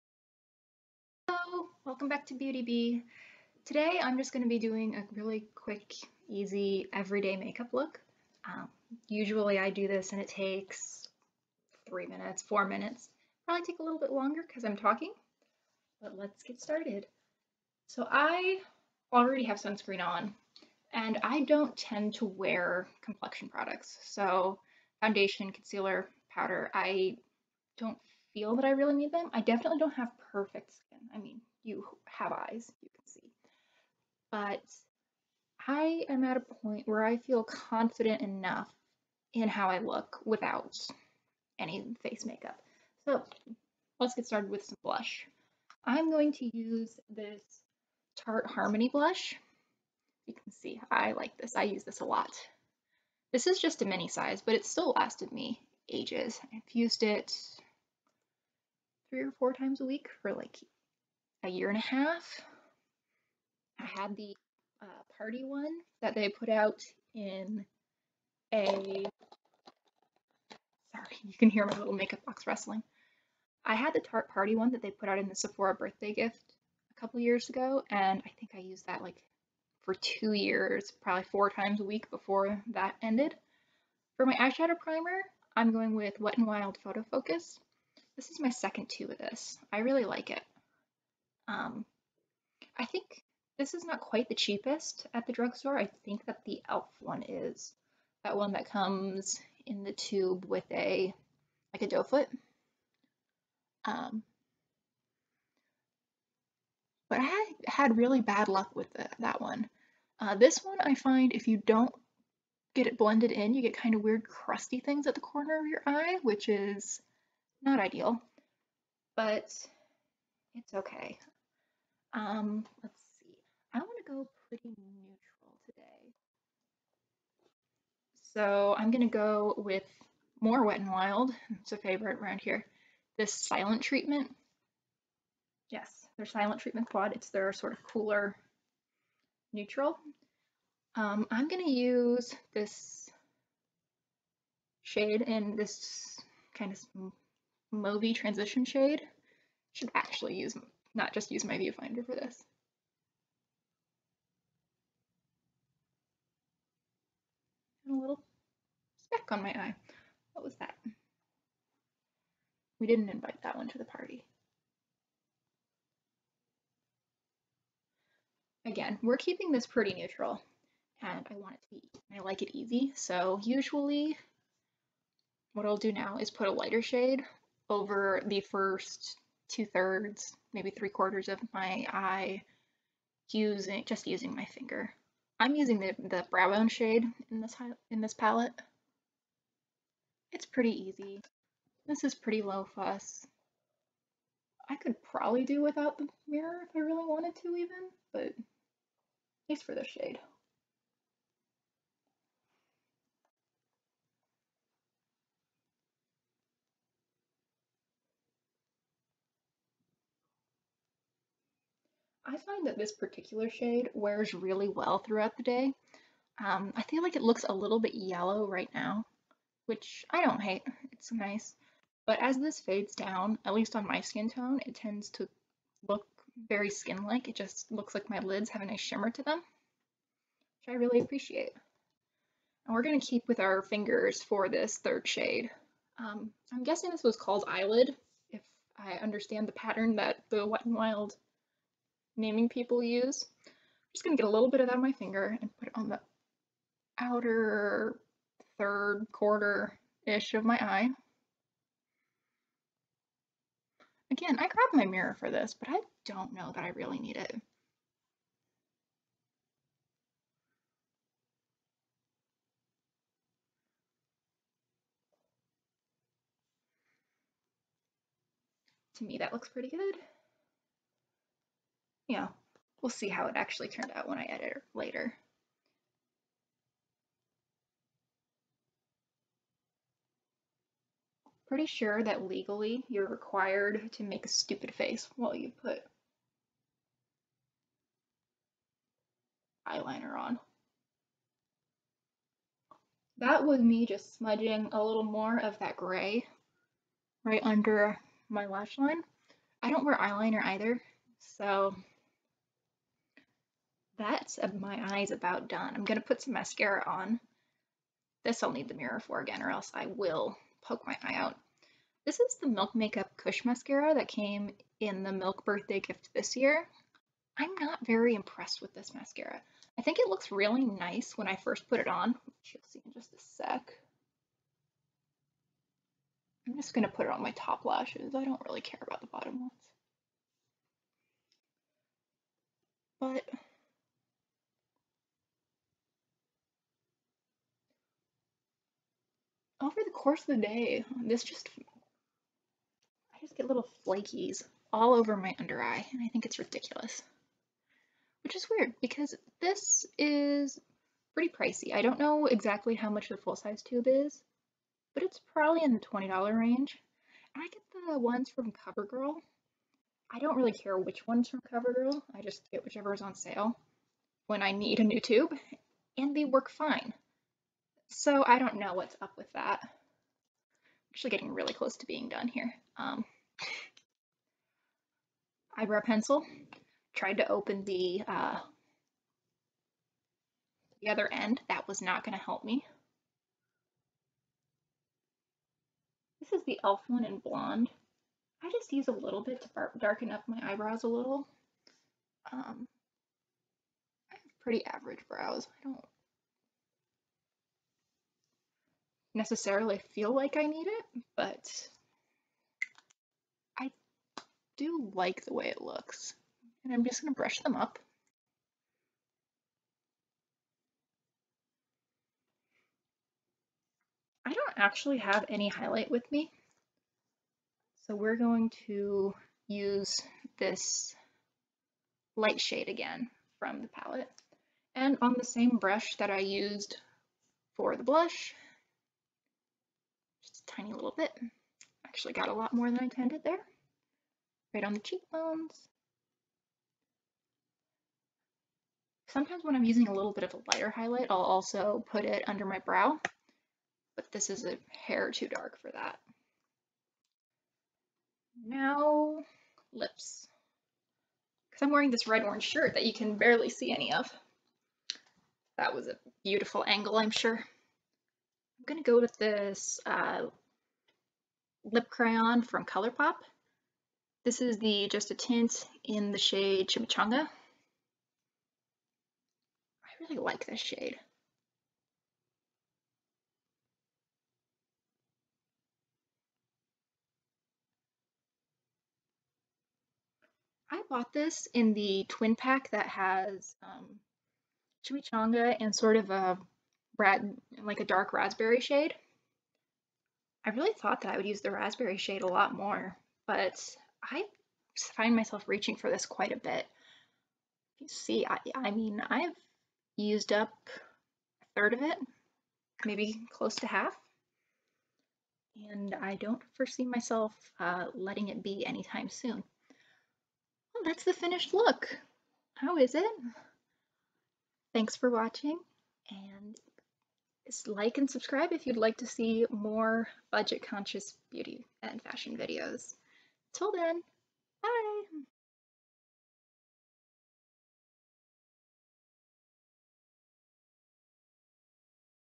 Easy Everyday Makeup Routine - 2010 YouTube Style
Featuring Wet n Wild and Sephora birthday gifts because I'm a poor grad student. It's 2010 style because I'm using my fingers, have terrible lighting, and everything together costs like $35 or something.